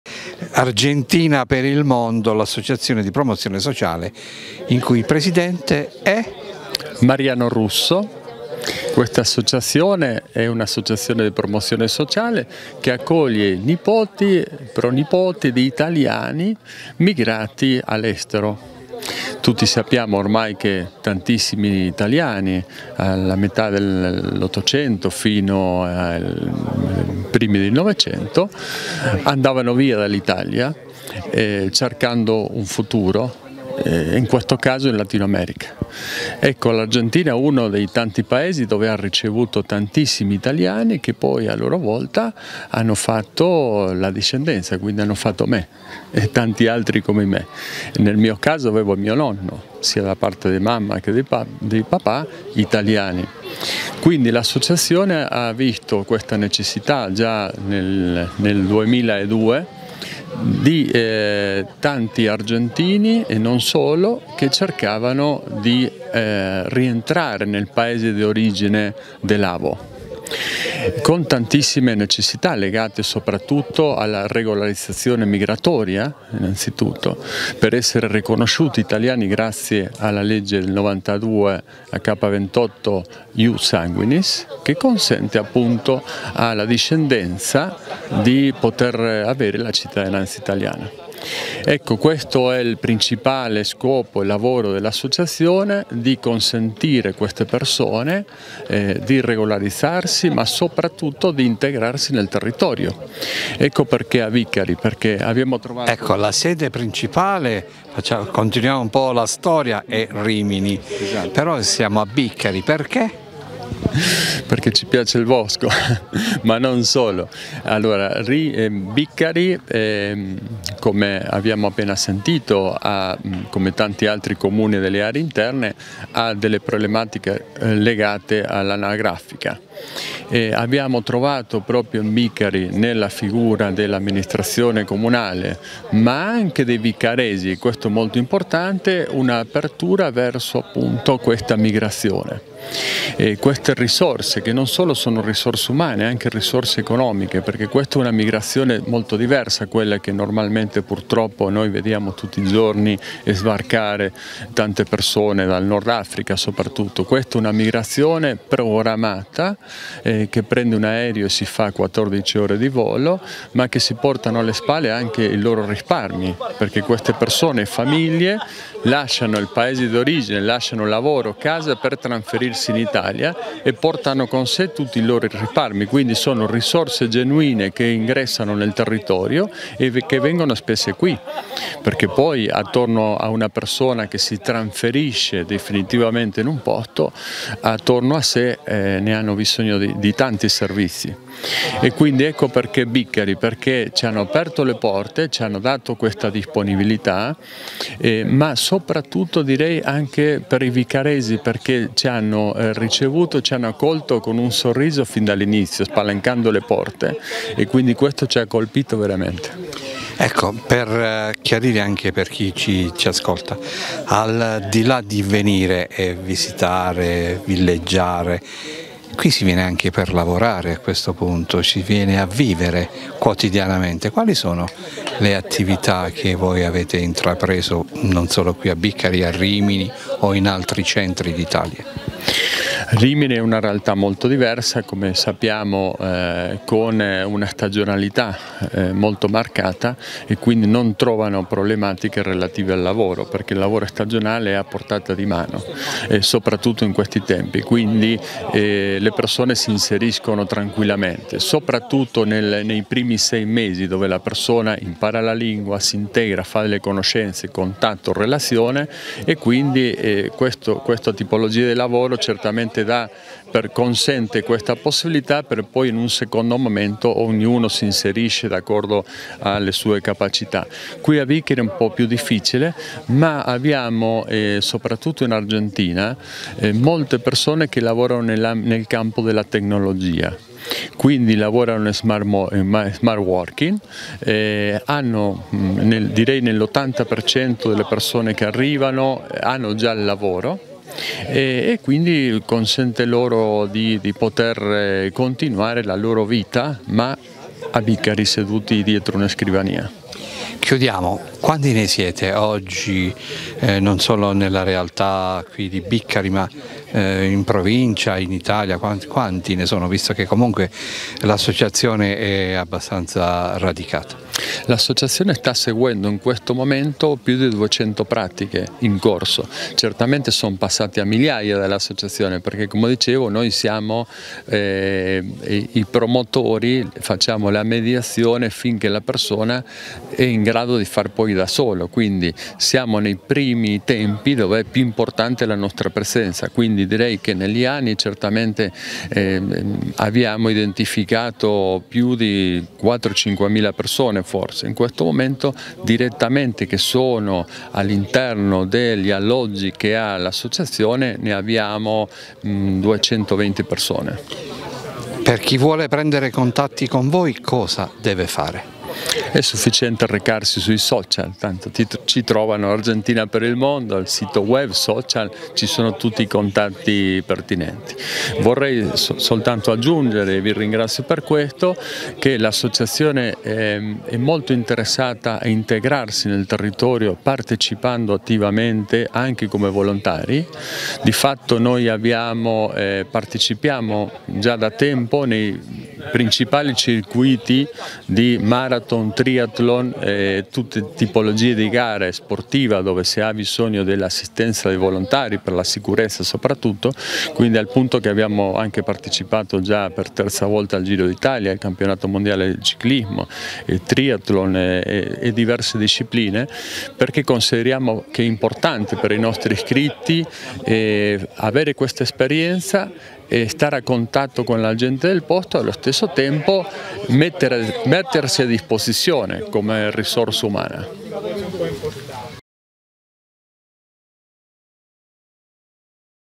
Argentini nel mondo anche a Biccari
La comunità degli argentini che vivono in Italia è sempre più numerosa, a Biccari abbiamo incontrato il presidente dell'associazione di promozione sociale Argentina per il Mondo, Mariano Russo, che ci ha raccontato della loro presenza in Italia, e dell'accoglienza ricevuta a Biccari.